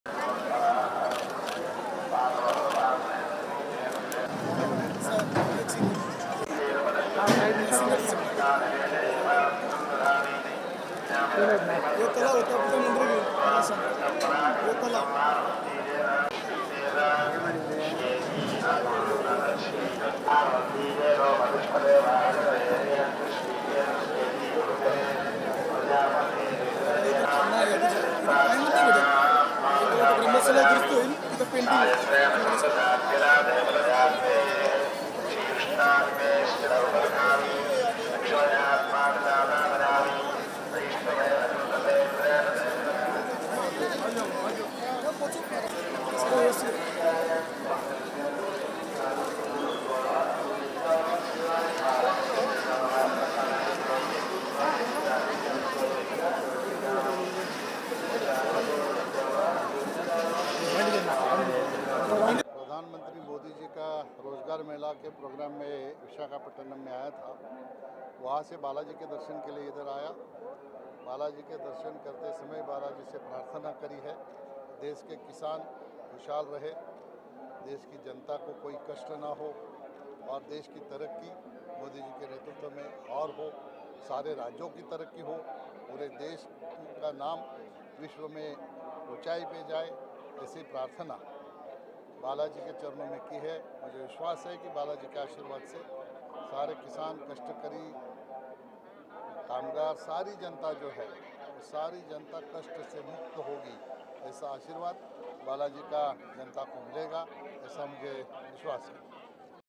Central Minister Darshana Jardosh Visits To Tirumala Devasthanam | Zup TV
Watch  Central  Minister Darshana Jardosh Visits To Tirumala Devasthanam | Zup TV